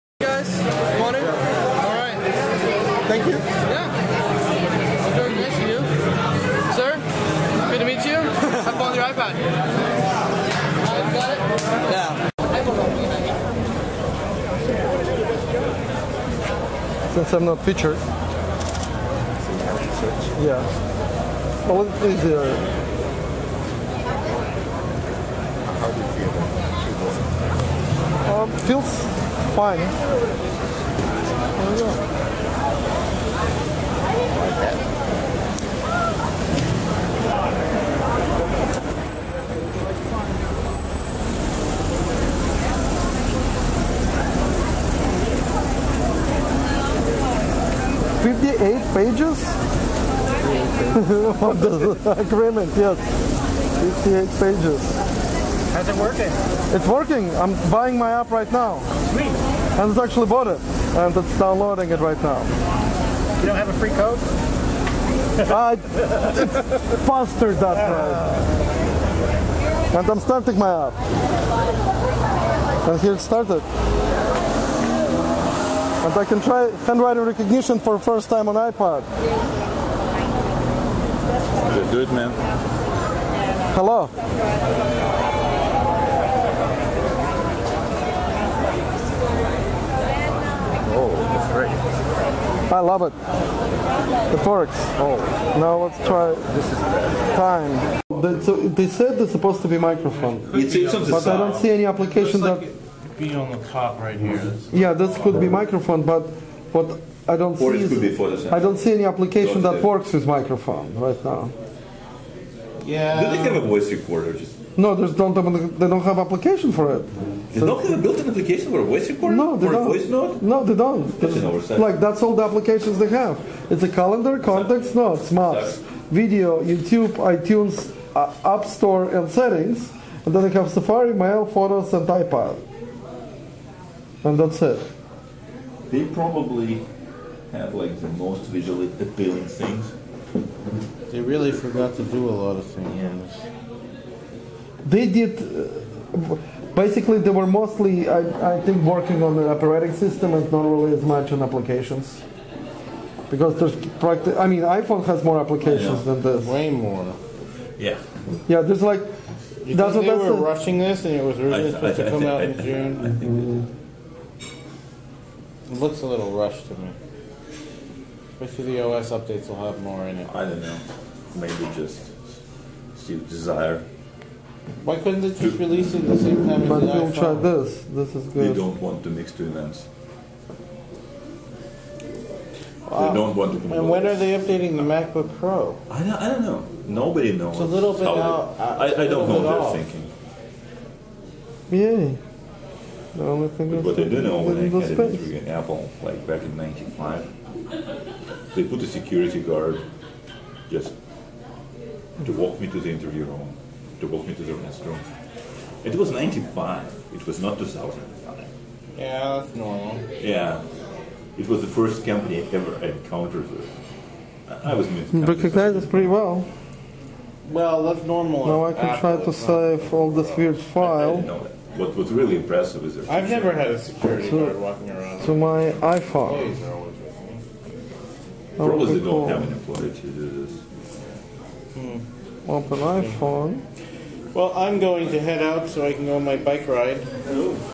WritePad For the iPad First Install
9 AM at the Palo Alto Apple store, WritePad runs for the first time on an iPad